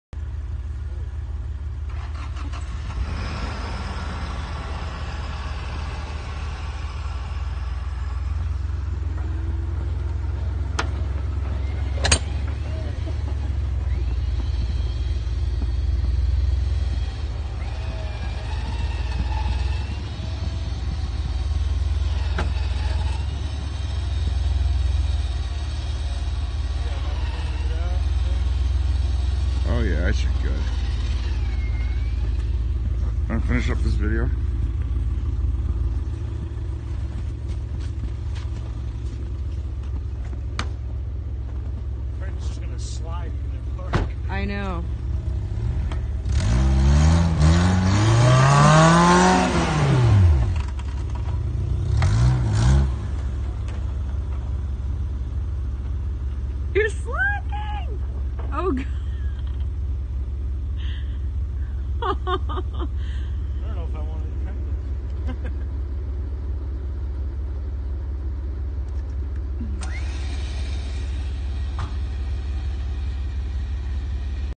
WINCHING JEEP OFF OF TRACKER
Here is The Black Taco winching the the Jeep off of the Tracker that is against the tree... Check out the tracker how slippery the Ice is, just slides down the trail which is what happened to the Jeep.. only a broken license plate bracket, the winch got pushed in a little too close to the radiator on a Tracker, It will have to go to the frame shop and have it pulled back out and the Jeep will need a new rear bumper.. THANKS to all my subscribers for watching Sunday Snow Wheeling ICE Wheeling Series Adventures with Sarah and The Southern Vermont Off-Road Associaton..